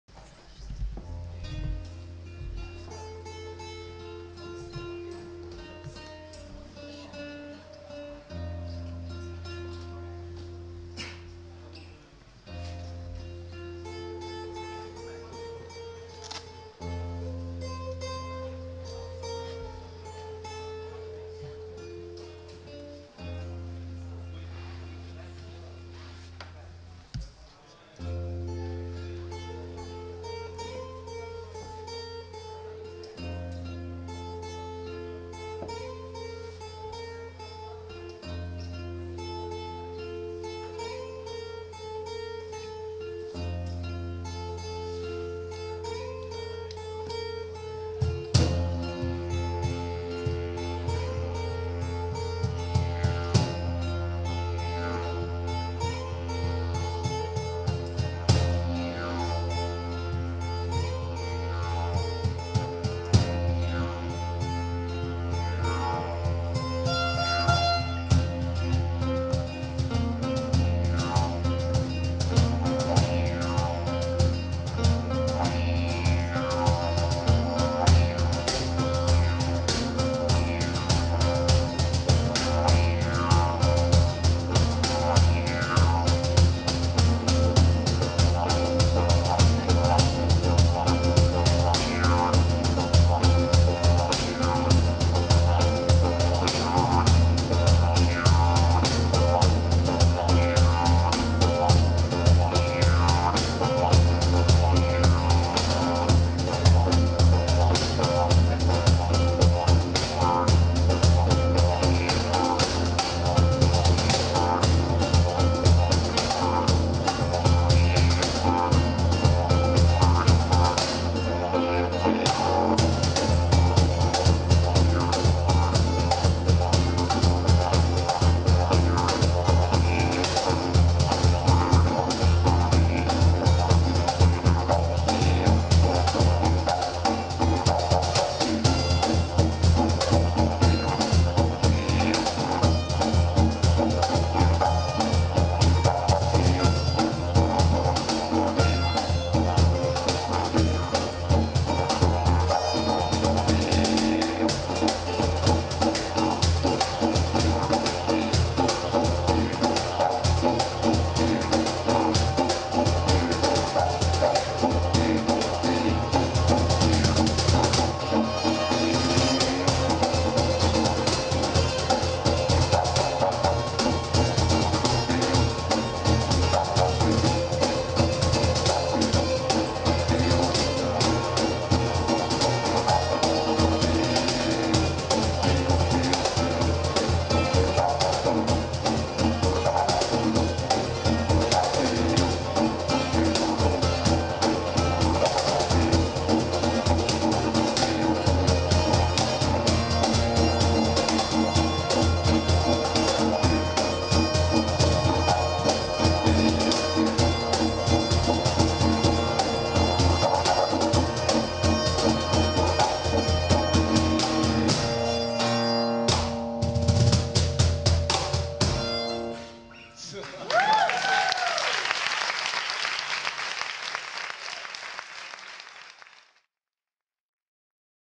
Gecko performing original music with special guest Devi on cajon 18/12/13
Gecko live @ Didgeridoo Breath